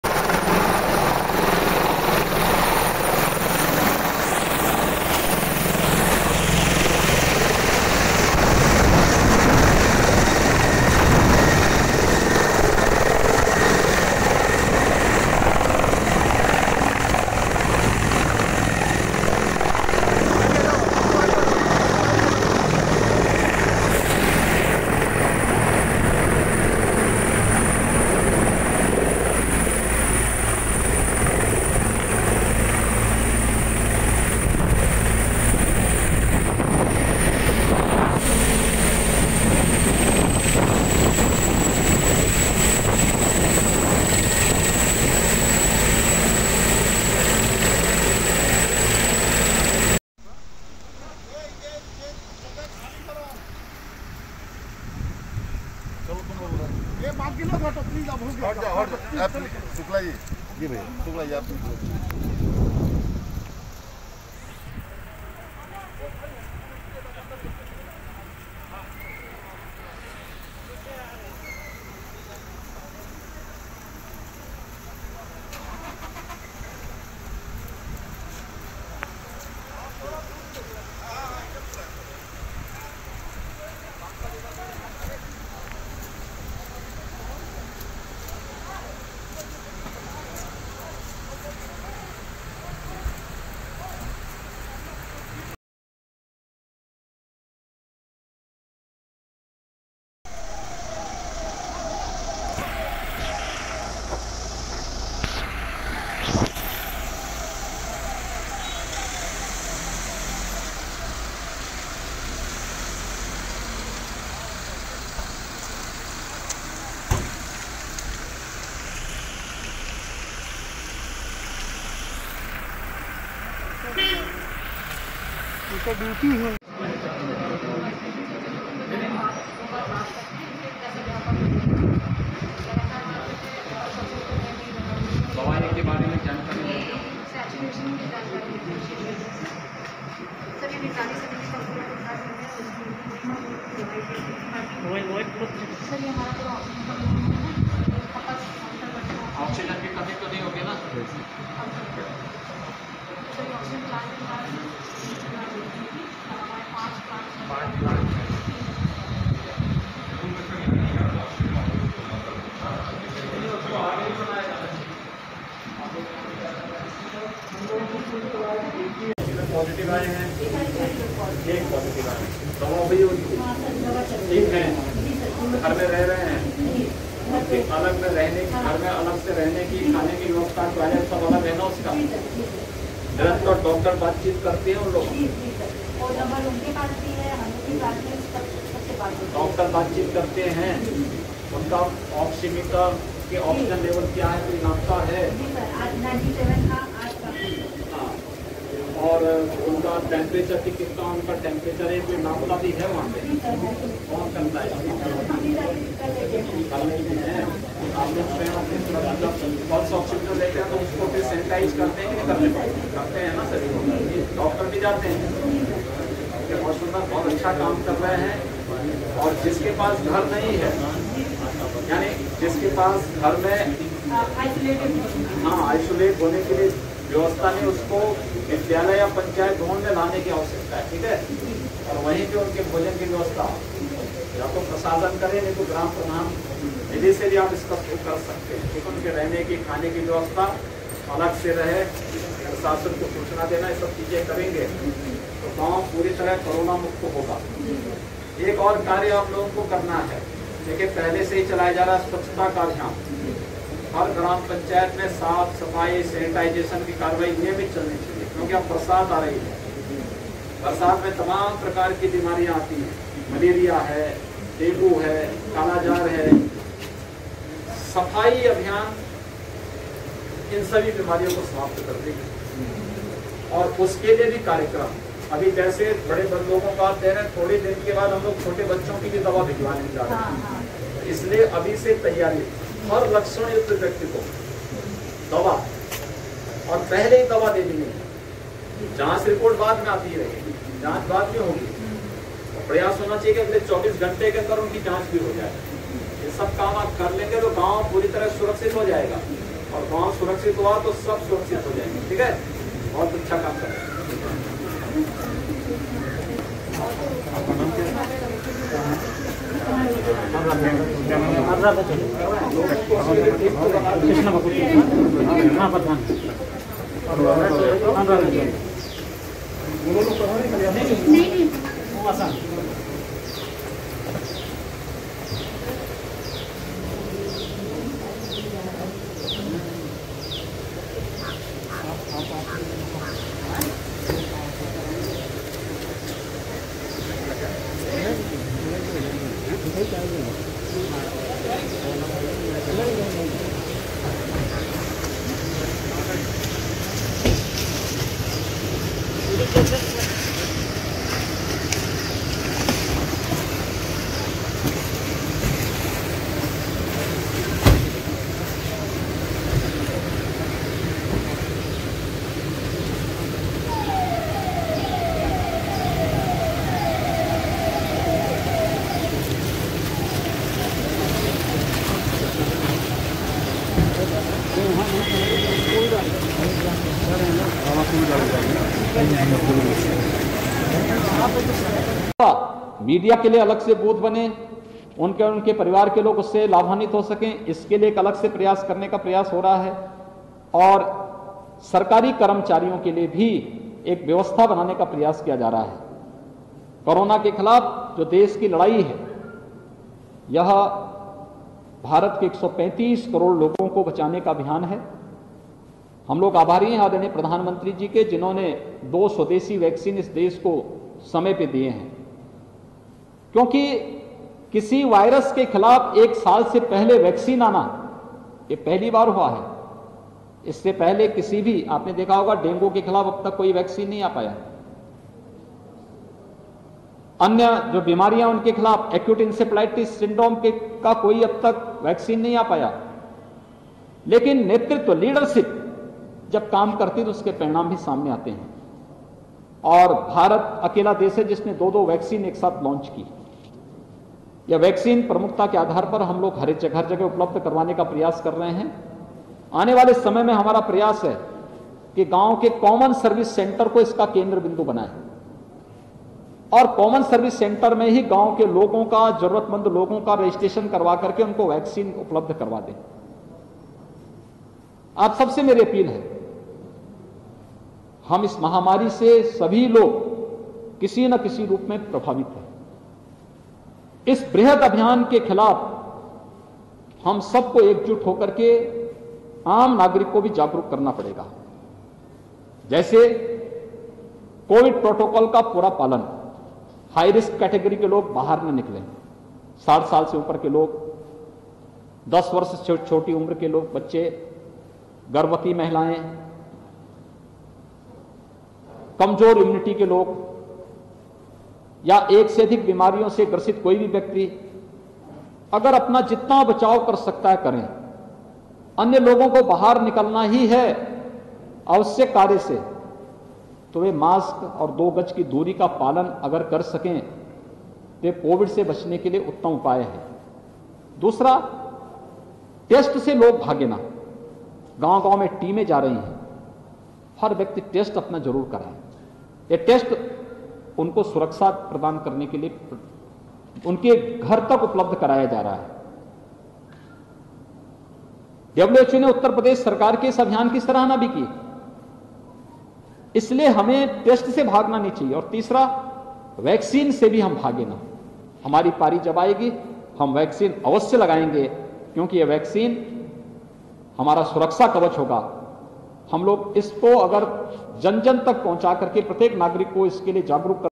UP CM Yogi Adityanath Basti दौरे पर||भारी बारिश में मुख्यमंत्री योगी जी बस्ती पहुंचे|| Aman Mishra
कोरोना के दौरान मुख्यमंत्री_योगी जी का बस्ती_दौरा।
भारी बारिश के बीच मुख्यमंत्री योगी आदित्यनाथ जी पहुंचे बस्ती।